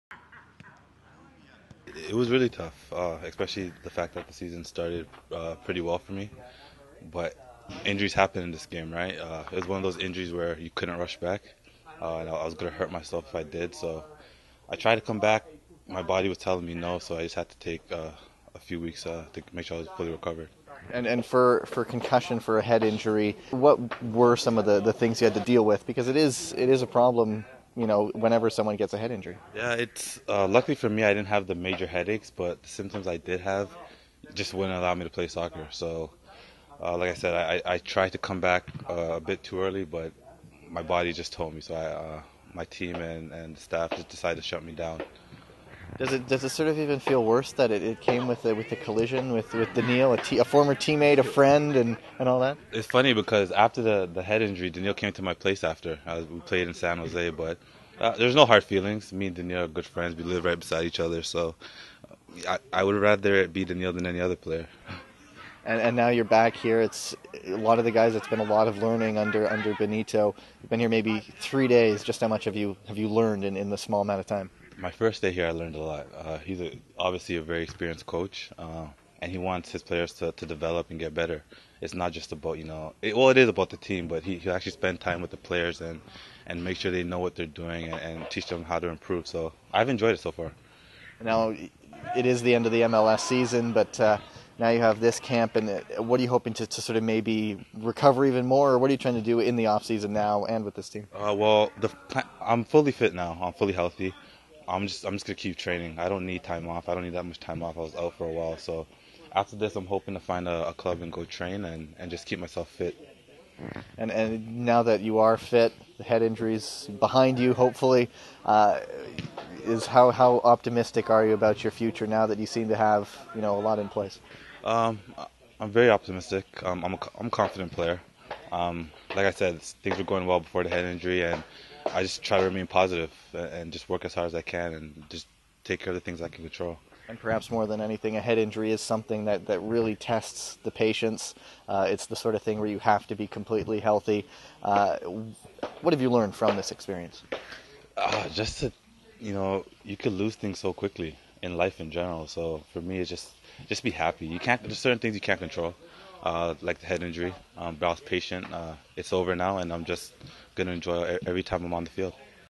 Canada MNT Nana Attakora eager to get back in the game
After more than six months away from the game recovering from a concussion, defender Nana Attakora has returned to Canada's Men's National Team fully recovered and eager to get his national team career back on track.